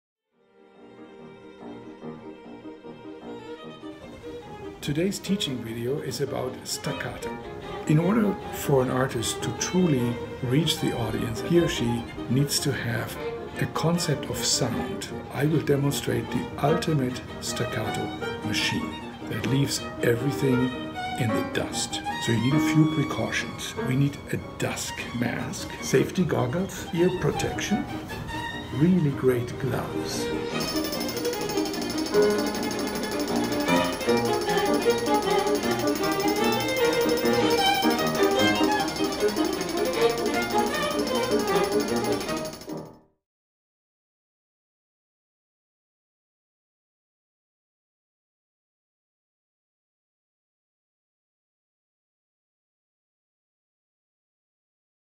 Learning Staccato in 45 seconds
Kurt Sassmannshaus teaches the ultimate up-bow staccato technique. With KayCee Galano, violin, and Rohan DeSilva, piano